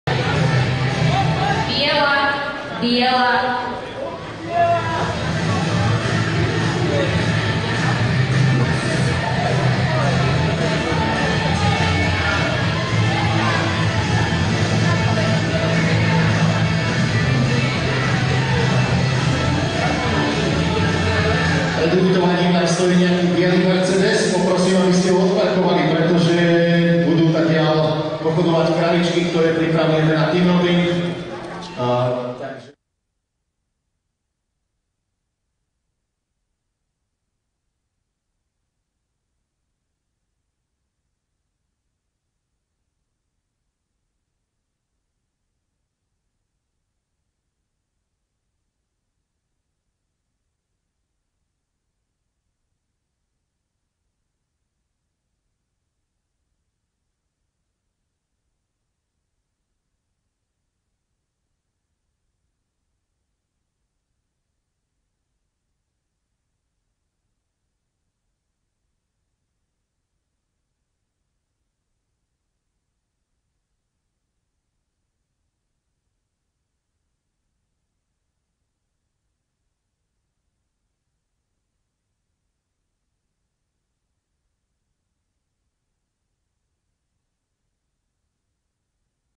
Team Penning - OPEN zalubenci a lucka 86,62 sek 3 cattle
1487 - Marek Žilinec.  - AH Smart Blues Peppy
SVK2391 - Lucia Kubačková.   -  Henessy Hidalgo Jet
SVK2392 - Soňa Púchovská.  -  Holly Delmaso
 Team Penning - OPEN
SK - Nemšova - 6.kolo-Ohradky Dobytok, 4.kolo-Lasa+Team Roping , 6.kolo Rychlostky